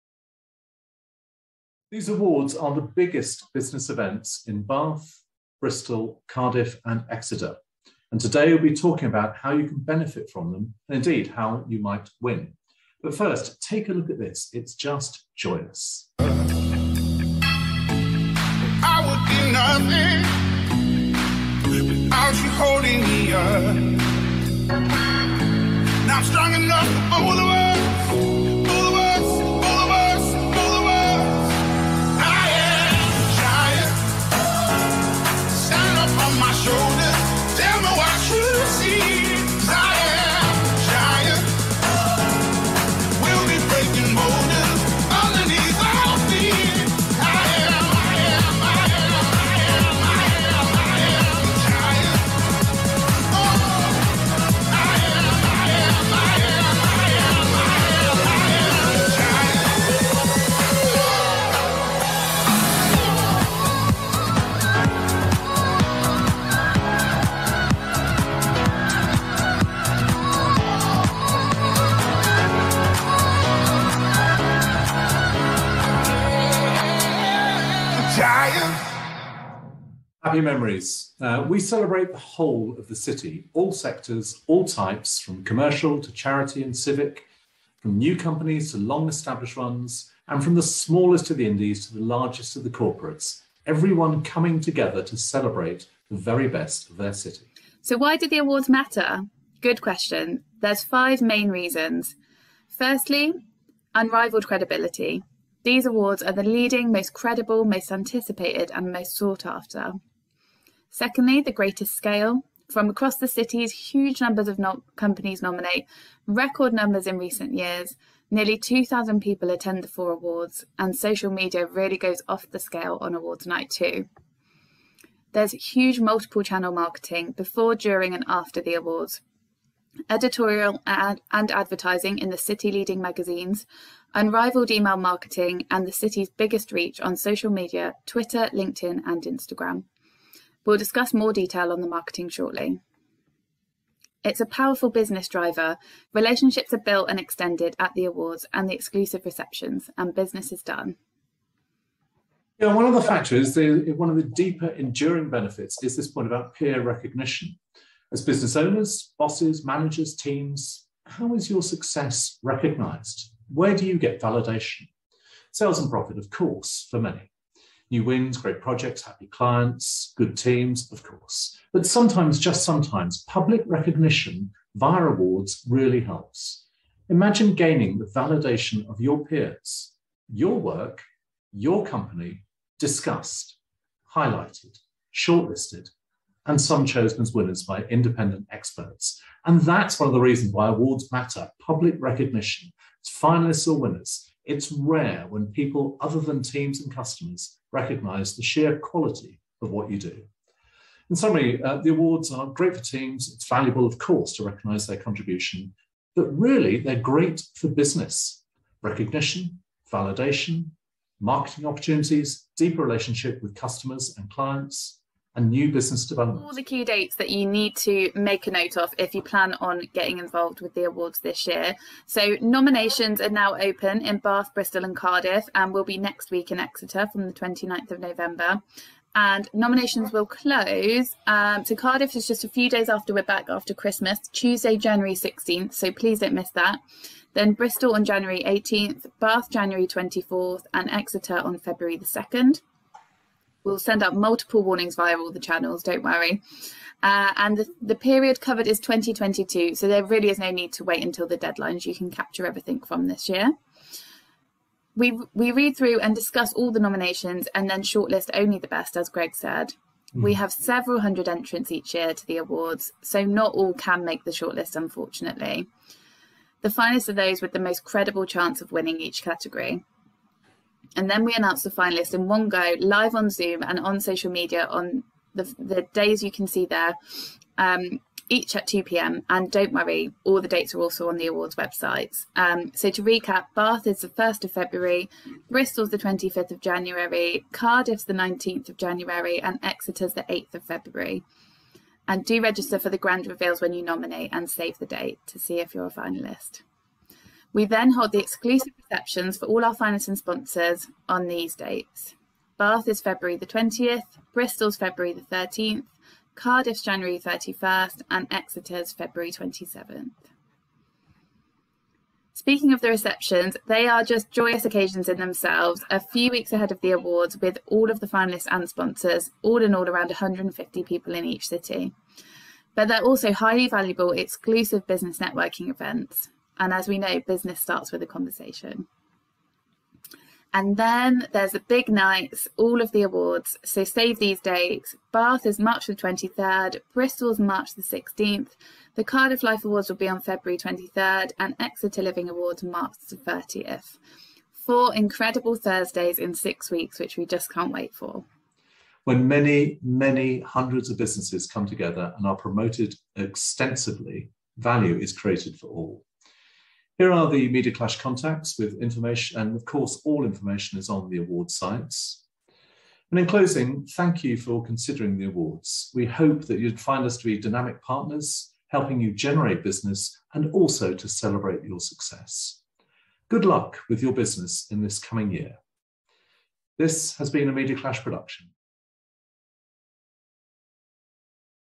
Why do the Bristol Life Awards Matter?
From the involvement of the greatest number of this city’s companies to unrivalled credibility and highly effective marketing campaigns, these Awards are powerful business drivers. They also give you the opportunity for peer recognition for your achievements.

Enter or sponsor the Awards to be part of the city’s biggest celebration!